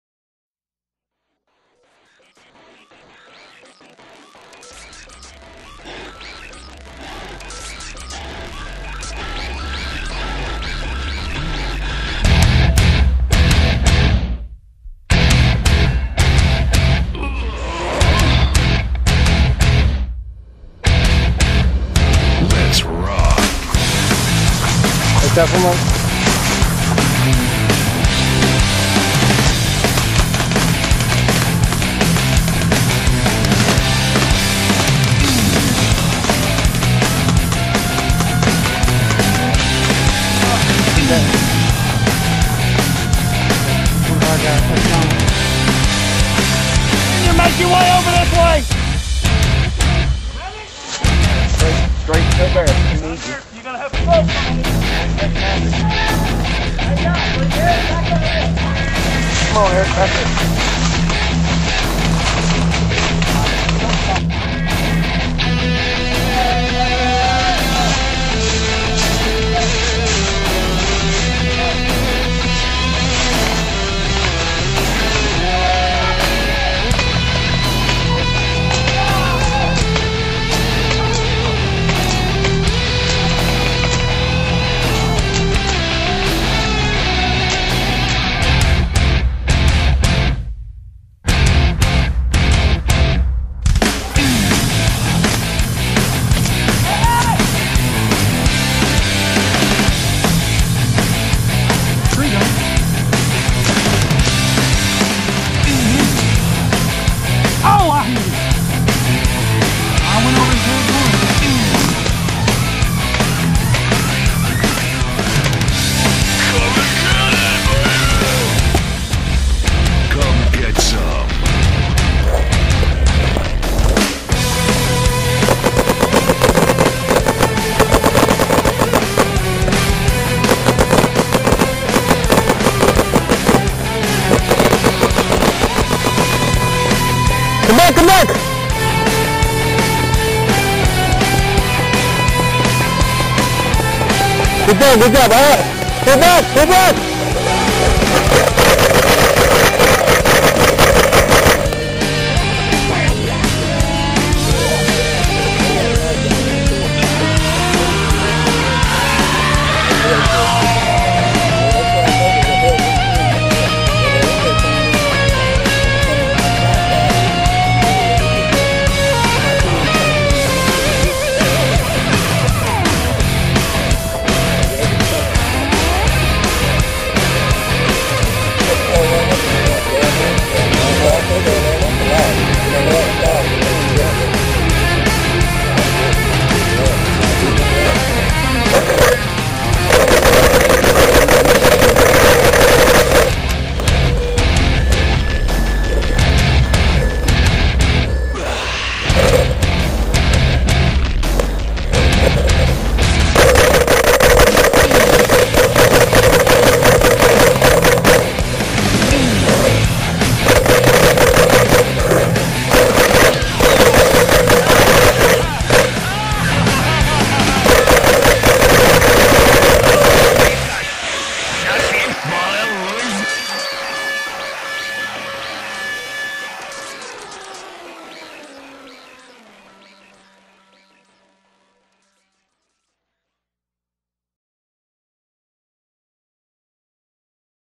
Prattville Paintball
Paintball game from 24 Feb 07 at Bobby's and 3 Mar 07 at Greg's.